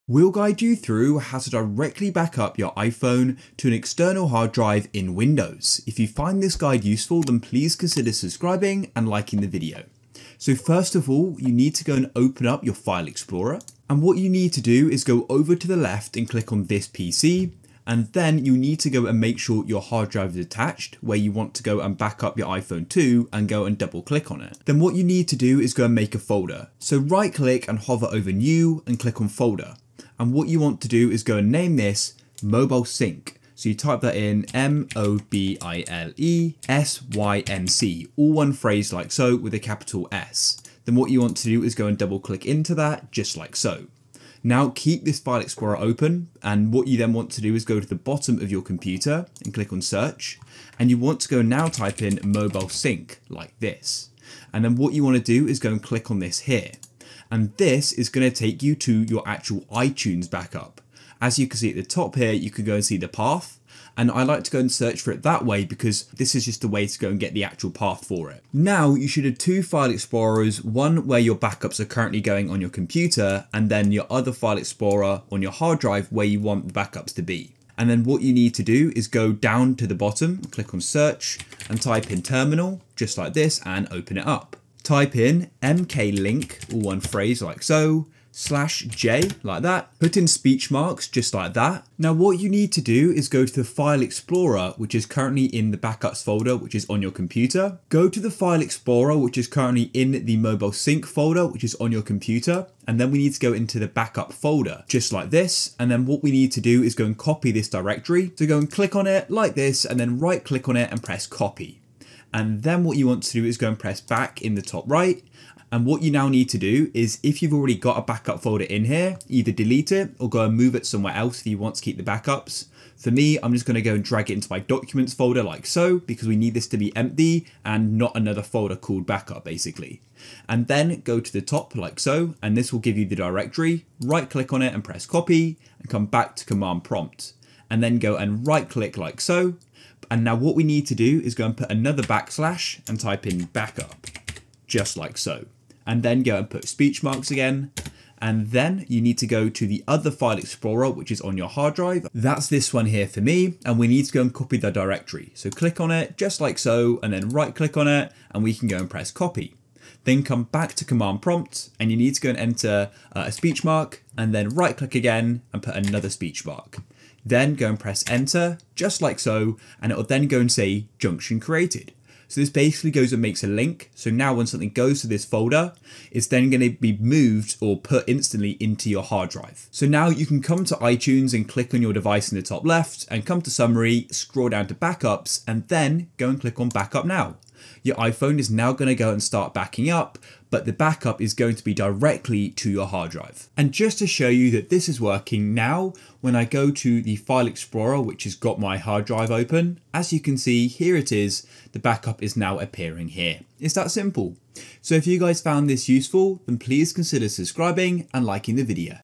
How To Back Up iPhone To External Drive On Windows (Direct) - Full Guide
Learn how to back up iphone directly to external hard drive on windows 11 and 10 in this video.

GuideRealm